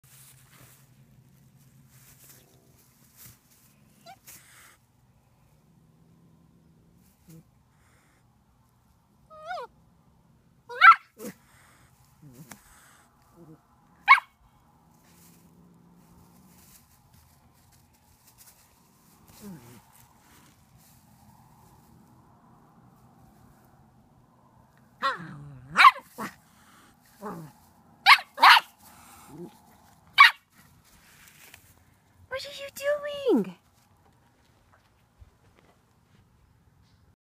Cute Puppy And Kitty Getting To Know Eachother
Cute puppy and kitty getting to know eachother. They are not too sure about this and the kitten keeps arching her back and hissing at the cute puppy. The puppy thinks the kitten is playing! LOL.